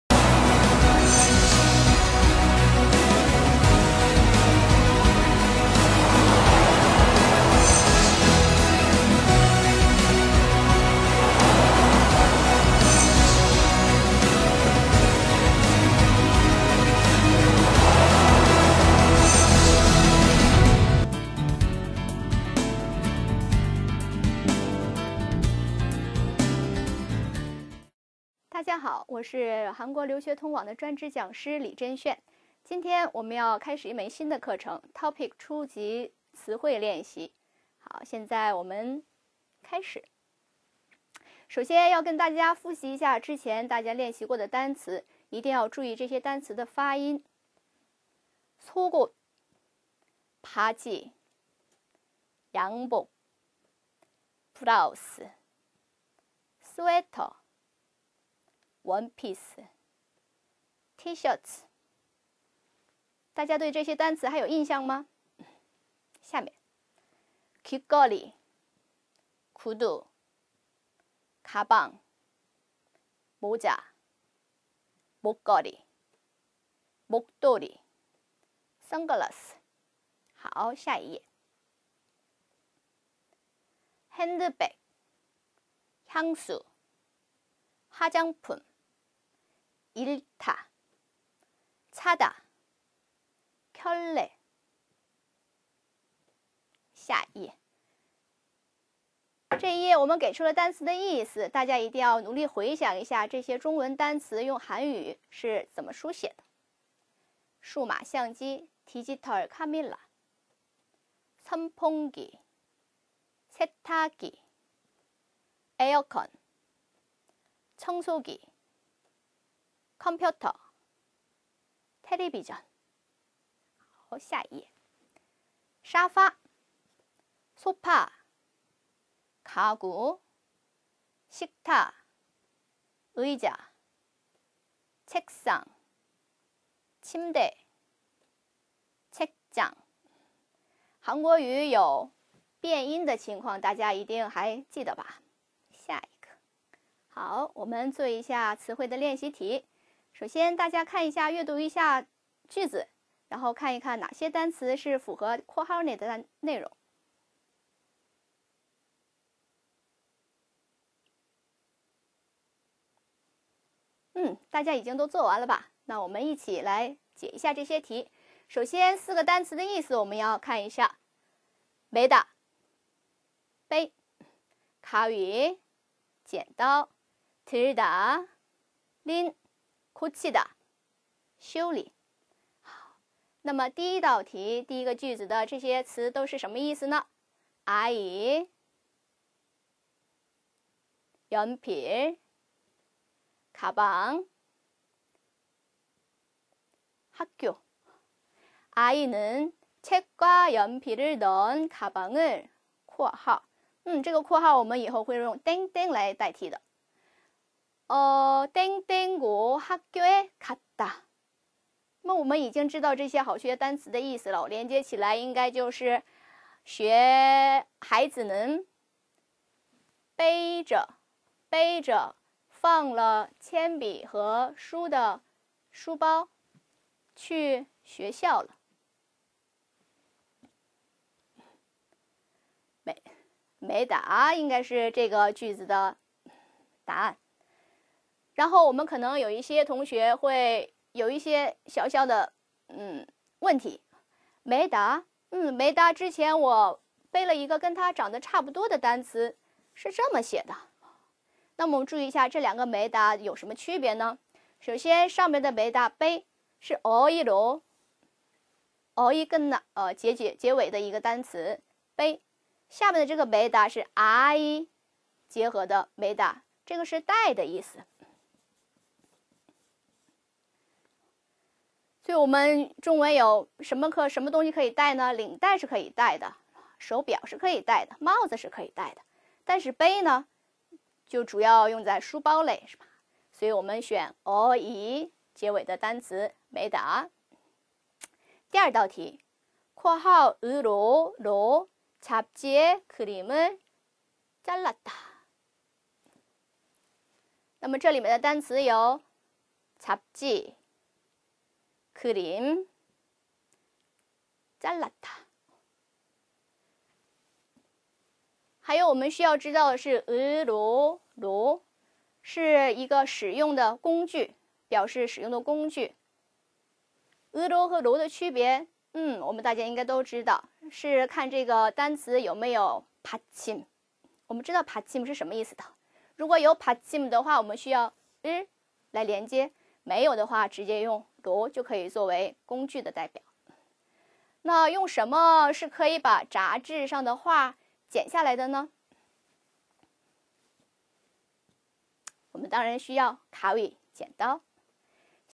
TOPIK 初级单词01
一个专门为学习韩国语的同学们准备的以真题练习为主的词汇练习课程。此教程由25节课构成，每节课大约20~25分钟。词汇内容涉及广泛并逐一进行分门别类，以便同学们能够融会贯通，进而更精确的掌握每个单词的含义及应用方法。韩国留学通网 韩语学习应用新鲜出炉！令您学习韩语更快捷更便利！